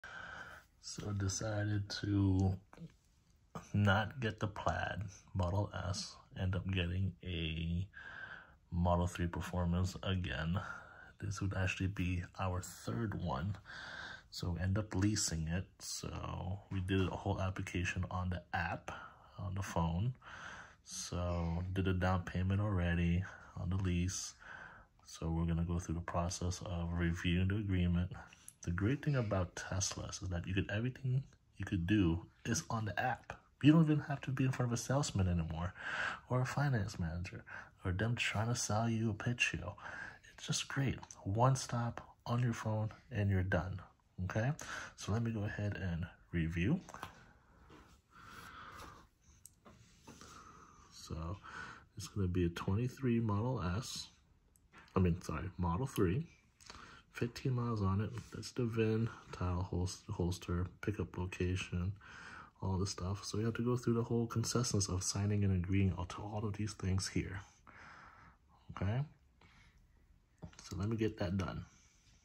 Incoming delivery 2023 Tesla M3P!Couldn’t say NO to Tesla’s free supercharging for 6 months!
Car rental: Click link to Book!

Charter: Click link to BOOK YOUR Boating Experience!